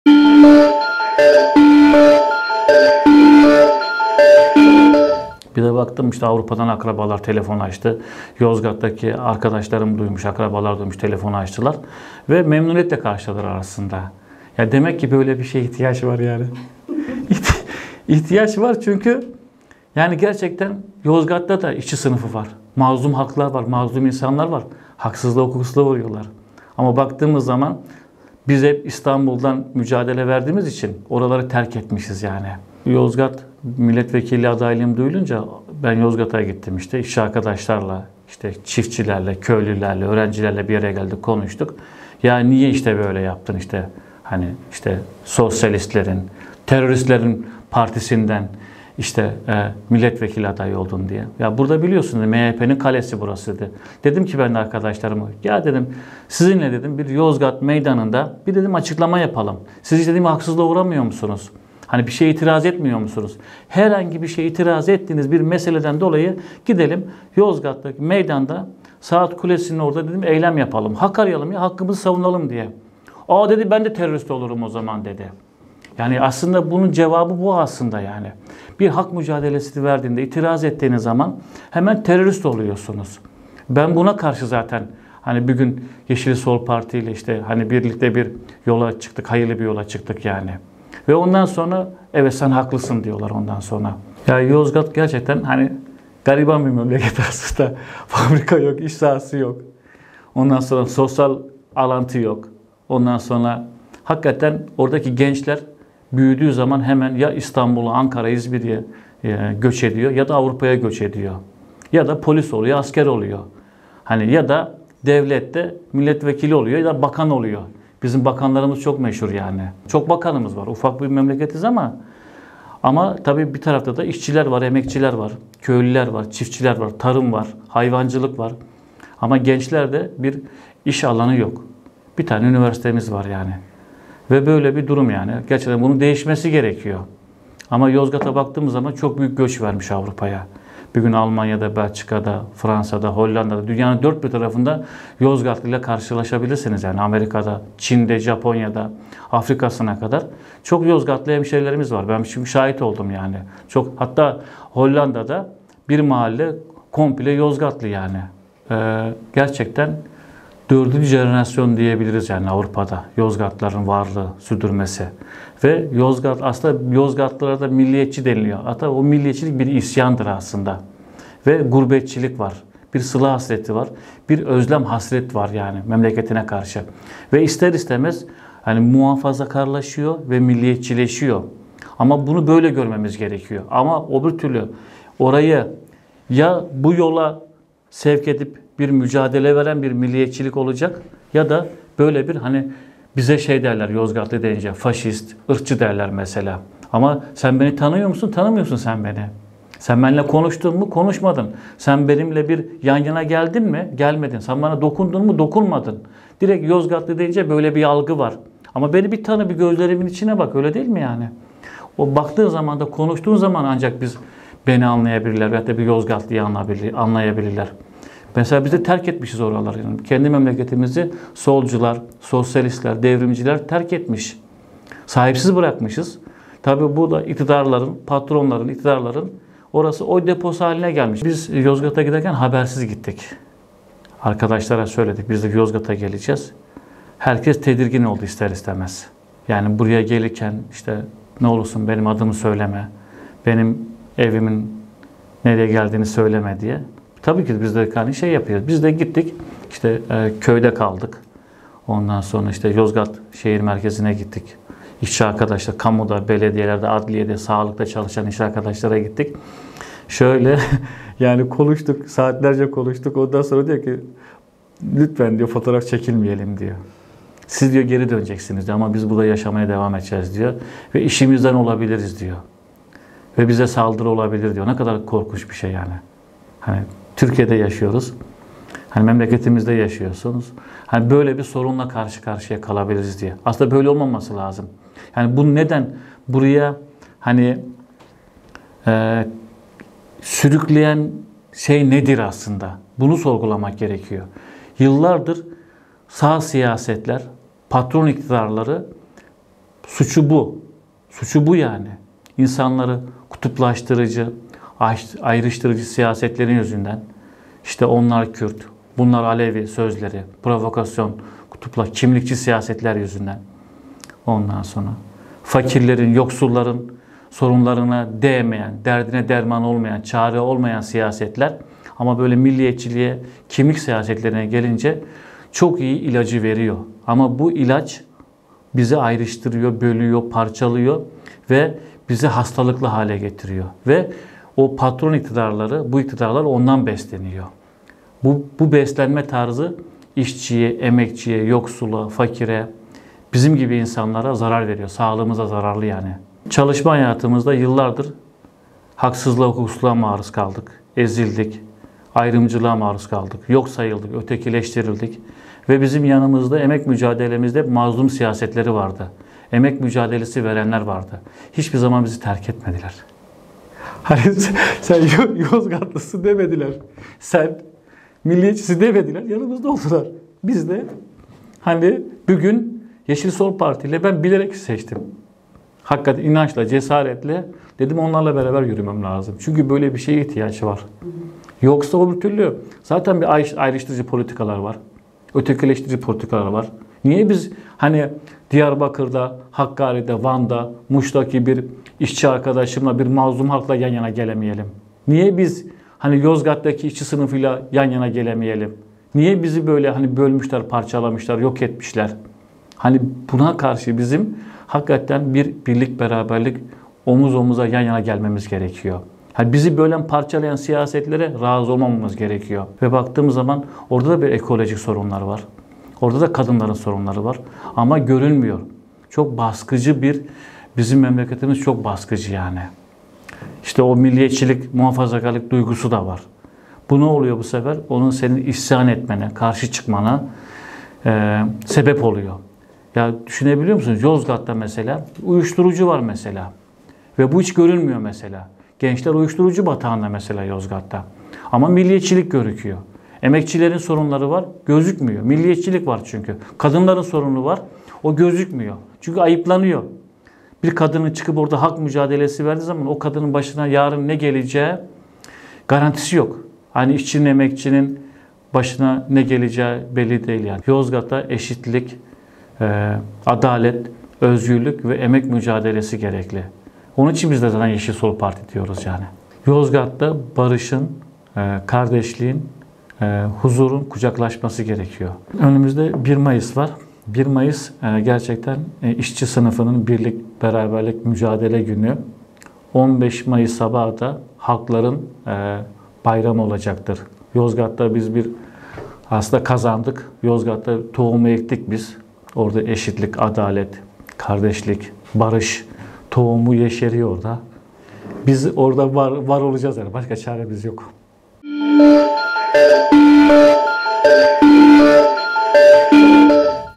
BAŞKA TÜRLÜ BİR YOZGAT MÜMKÜN | YEŞİL SOL ADAYI CEMAL BİLGİN ANLATIYOR
Dindar ve milliyetçi ailesi tarafından ‘şükret, sabret, aman oğlum amirlerine karşı çıkma’ diyerek büyütülen Cemal Bilgin, şimdi Yeşil Sol Parti’nin Yozgat milletvekili adayı.

Onu, ‘sağın kalesi’ olarak bilinen memleketi Yozgat’tan hem de Yeşil Sol Parti’den milletvekili adayı olmaya götüren hikâyesini kendisinden dinledik. Söz, “Yozgat’ta biz aslında şimdiden kazandık. Eşitlik, adalet, kardeşlik, barış tohumu yeşeriyor orada. Yozgat’ta bu tohumu ektik. Biz orada var olacağız, başka çaremiz yok” diyen Bilgin’de…

❝Sözün yükünü taşır❞